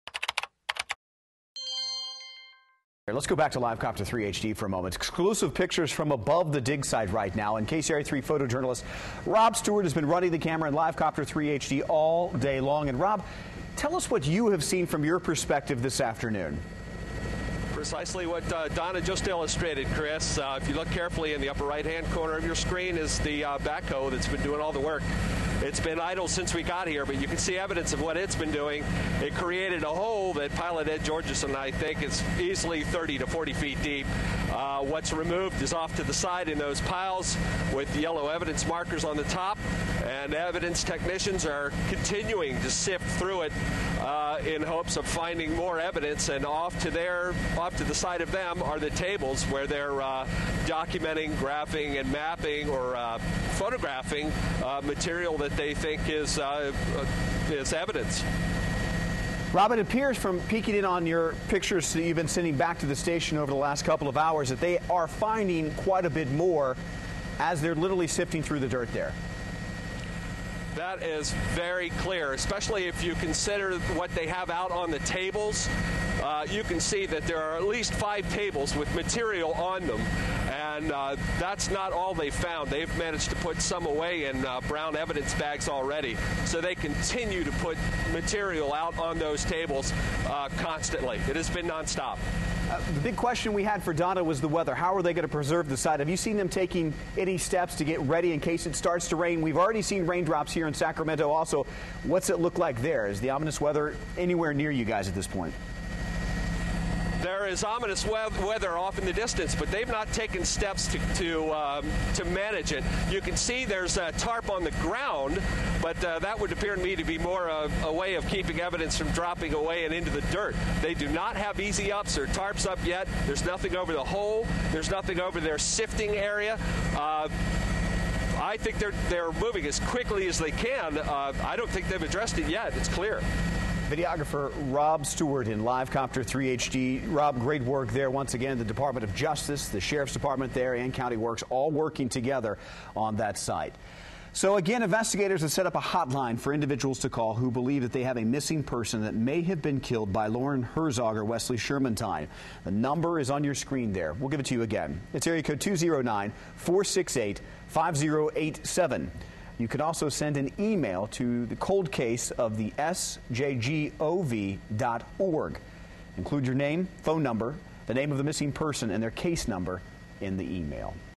LiveCopter 3 Aerials Of Linden Dig Site
LiveCopter 3 shows aerials of Linden dig site where search crews have uncovered bone fragments, clothing, and jewelry.